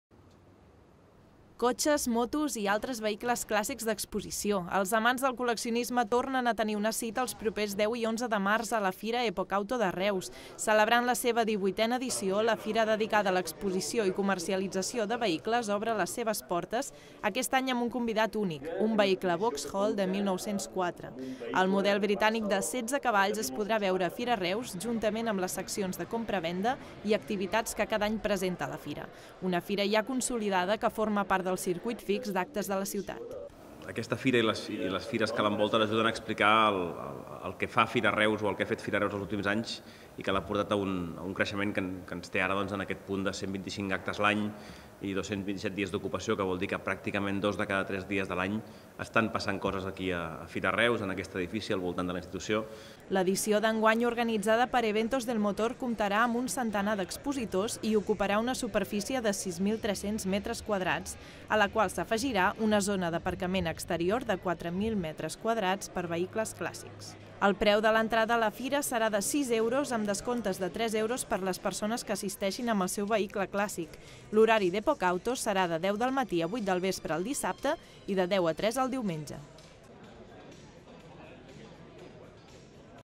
La 18a edició d’Epocauto s’instal•la aquest cap de setmana a firaReus
Epocauto celebra la seva divuitena edició a Reus. El saló dedicat a cotxes i motos clàssiques de col•lecció s'instal•larà els propers 10 i 11 de març a les instal•lacions de firaReus. Enguany comptarà amb una peça única: un vehicle Vauxhall de 1904.